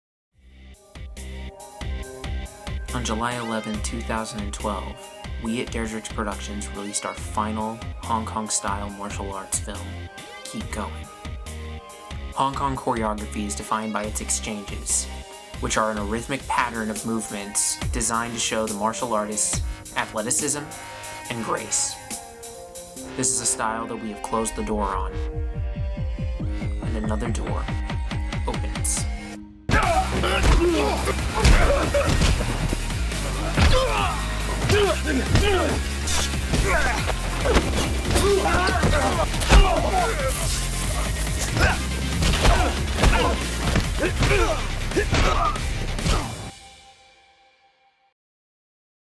Dardrex Productions NEW Fight Choreography Style Promo
Preview of a bunch of test fights showing Dardrex's new style of fight choreography now that Keep Going has closed the door of HK Style Choreography. Check back this weekend for the full fight scenes.